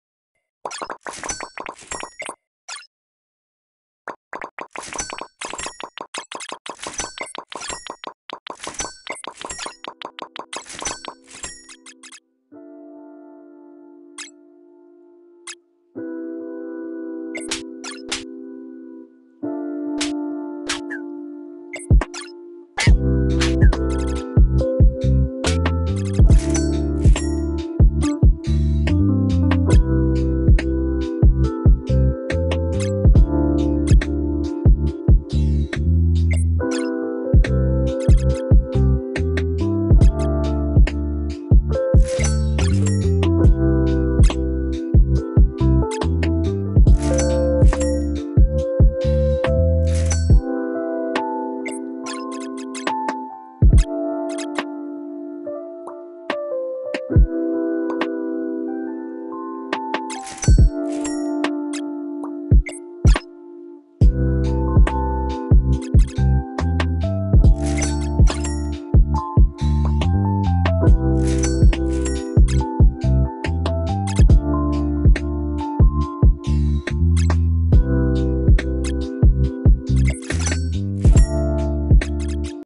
Building Mega Store from Zero to Hero - eps 1 | WTF!! Why there are fights?
Games building Mega Store from zero, look at them there are fighting over the queue, it's so funny :lol